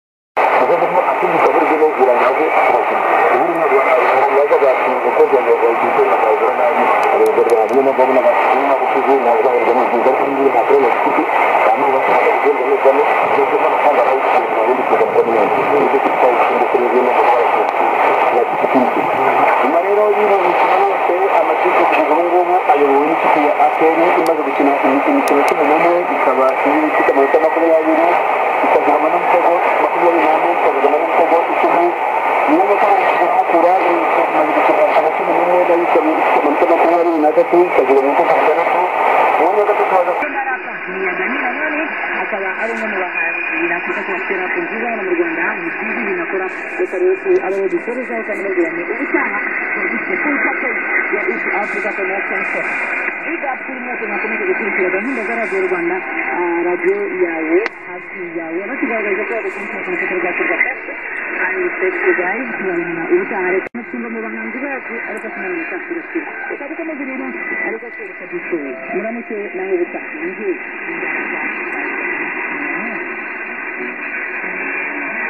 Remembering Radio Rwanda 6055 kHz (old recordings)
I have taken a couple of clips from my old videos which I have posted previously to remember Radio Rwanda. Thanks to senderjaeger for pointing out that it was Deutsche Welle who took the decision to pull the station as well as the Kigali relay transmitter off air. Hopefully R.Rwanda will return to the air some day through a new relay. Notice that it suffered less from QRM a few years ago than recently due to nearby stations.